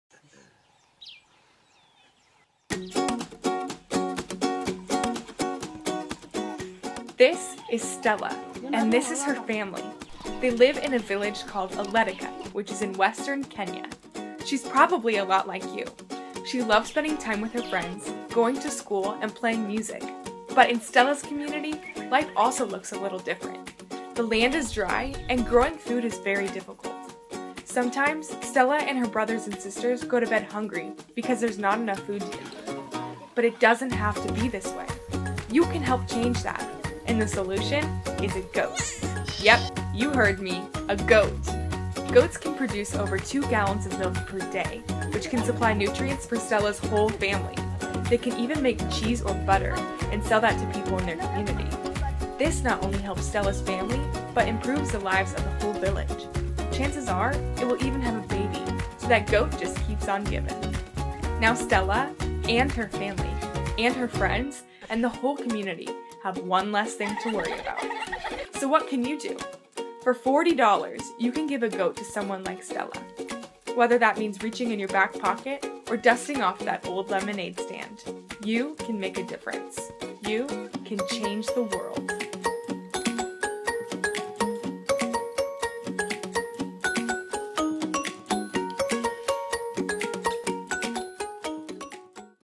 The Goat Project
Meet Stella, a spunky and talented girl from Oletukat, Kenya. Stella lives in an area that is very dry and growing food is very difficult. Through the Hope Venture, Stella was able to receive a goat, which provides food and income for her and her family. To find out more visit: